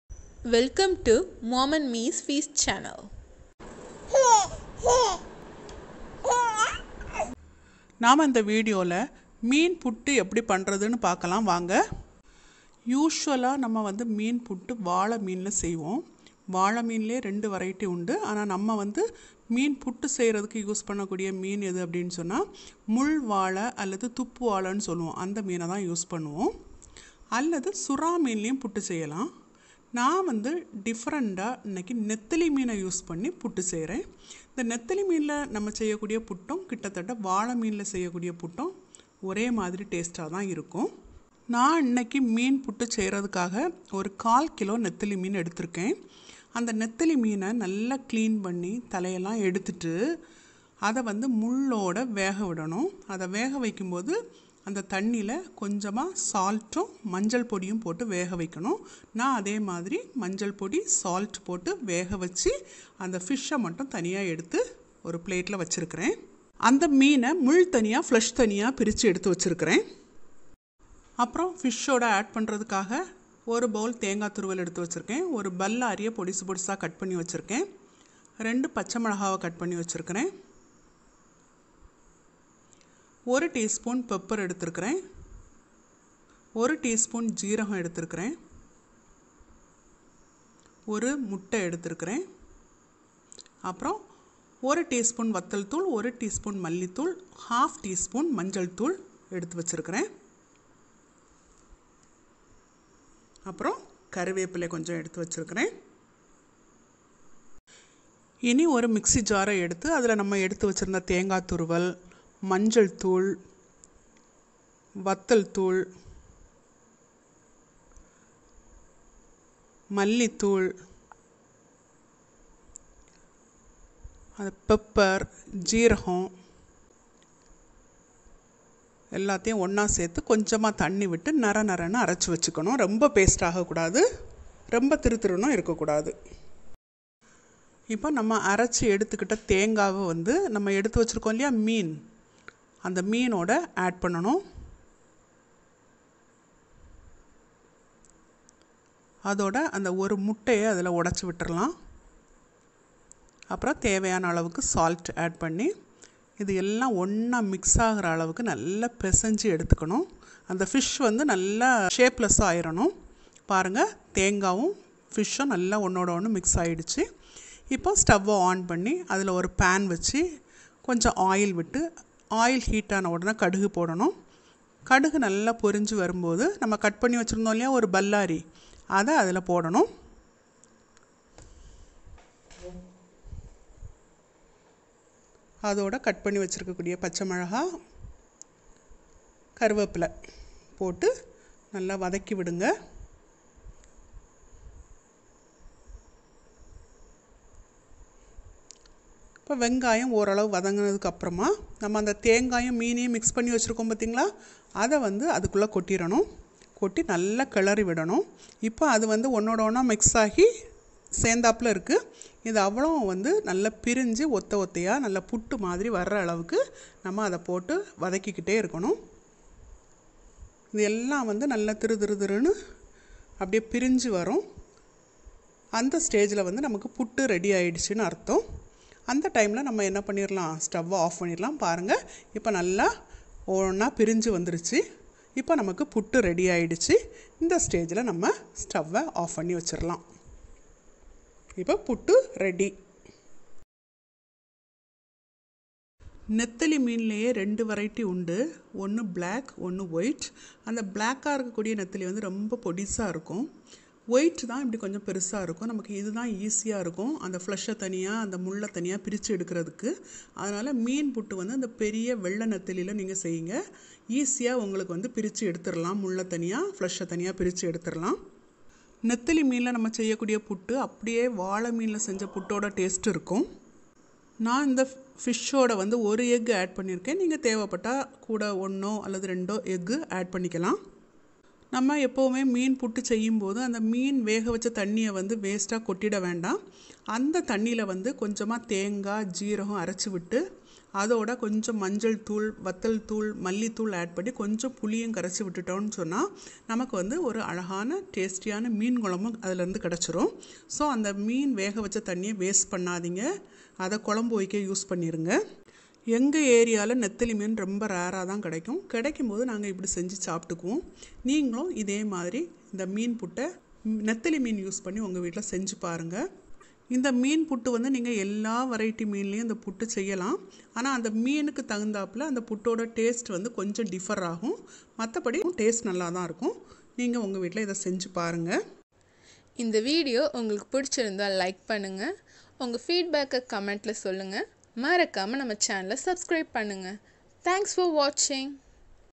நெத்திலி மீனில் கூட இப்படி செய்யலாமா ? / Anchovy Puttu #95#. fishputtu momandmesfeast#
Ingredients :
Nethili fish / Anchovy - 1/4 kg
Grated Coconut - 1 bowl
Egg - 1 or 2
Curry leaves
Oil
Mustard seeds - 1 tsp
Salt